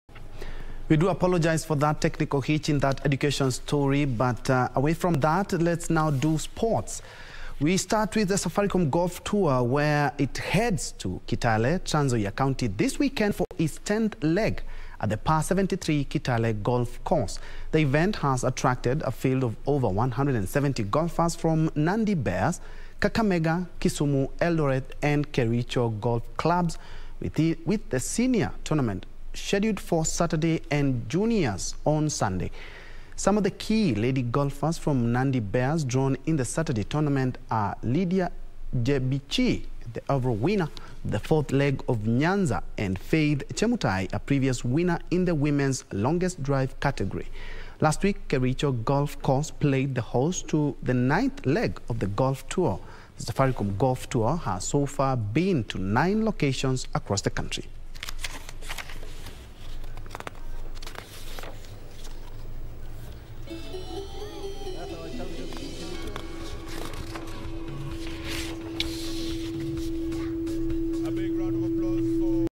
The Safaricom Golf tour heads to Kitale this weekend
The Safaricom Golf tour heads to Kitale, Trans Nzoia County this weekend for its tenth leg at the par 73 Kitale Golf Course.